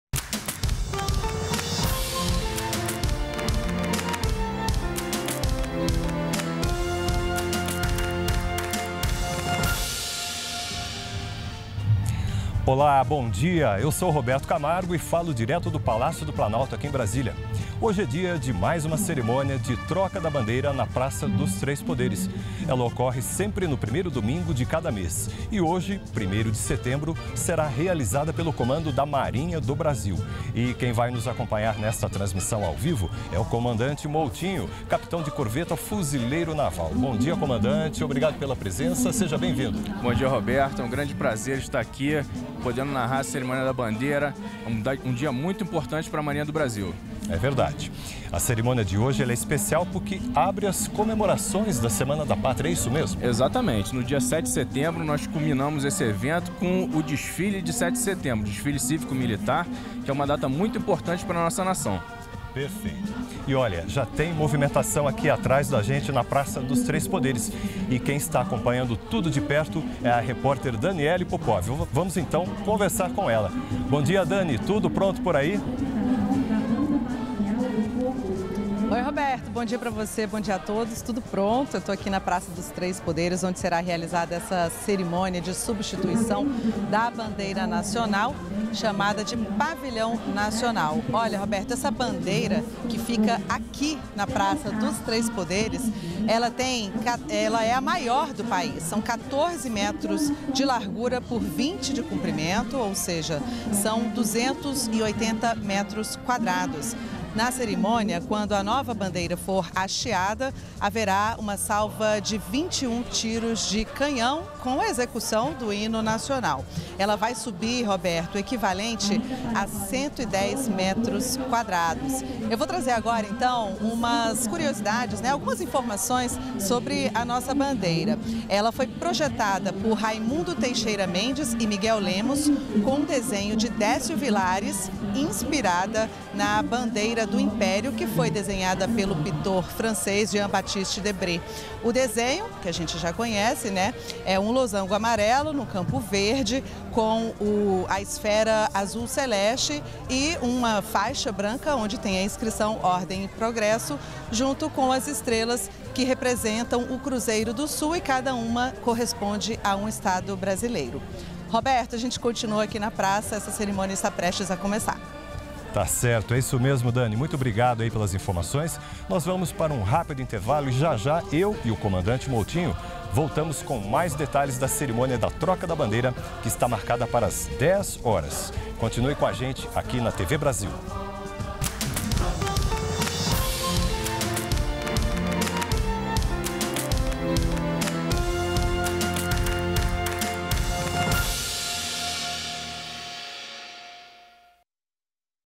Troca da Bandeira é comandada pelo Comando da Marinha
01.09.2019 - Acompanhe a Cerimônia de Troca da Bandeira, na Praça dos Três Poderes. A solenidade ocorre sempre no primeiro domingo de cada mês. Neste 1º de  setembro foi realizada pelo Comando da Marinha do Brasil.